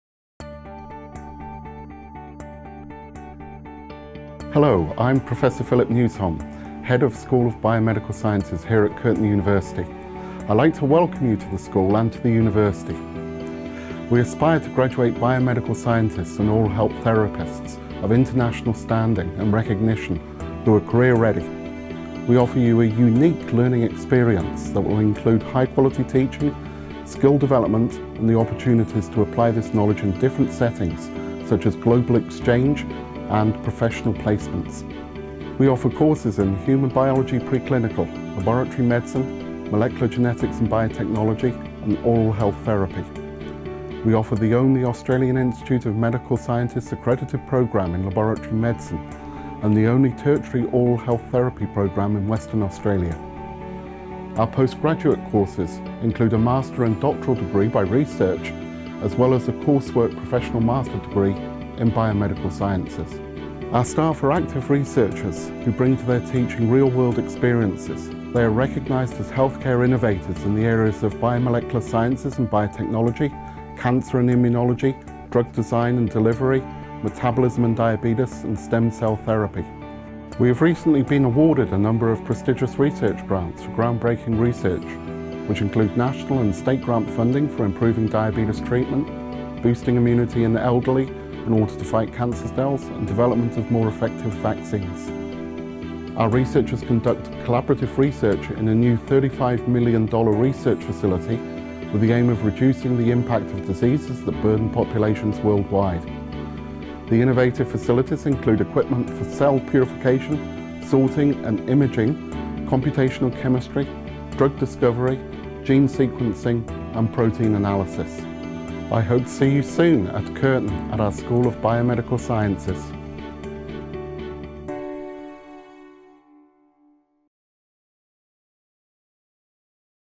Study Biomedical Sciences at Curtin Uni
Biomedical science is a rapidly expanding area encompassing a wide range of disciplines all over the world. Our courses provide a fundamental basis for all studies in the health and allied health professions, producing graduates that are internationally recognised and career ready. You'll also obtain practical, hands-on experience learning in high quality laboratories and facilities.